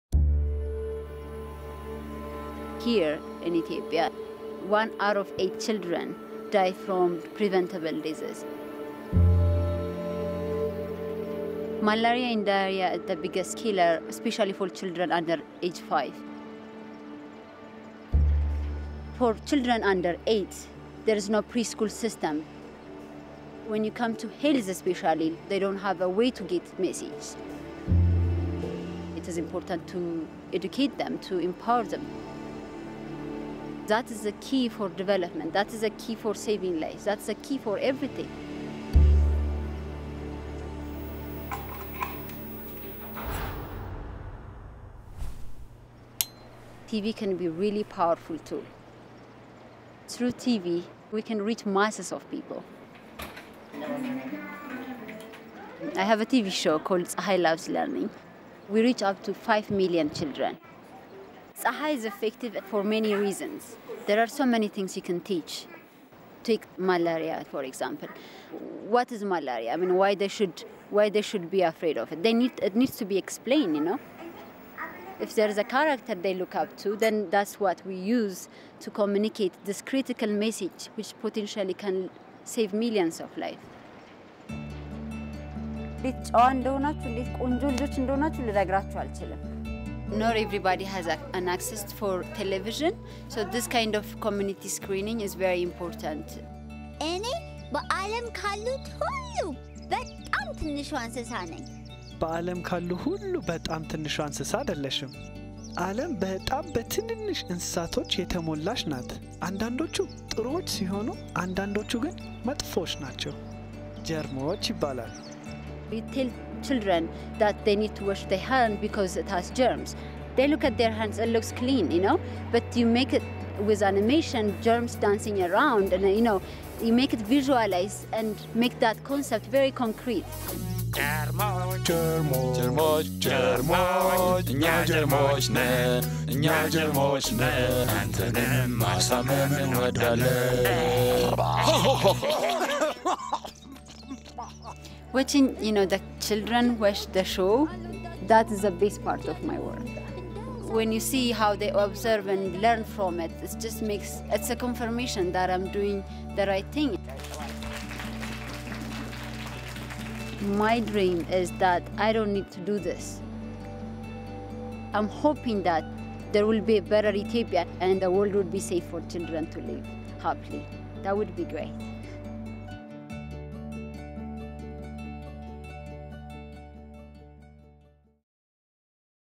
Rolex Young Laureates Programme: Bruktawit Tigabu
Saving Children Through Health Education
Bruktawit Tigabu explains how her animated television program, "Tsehai Loves Learning" reaches young children.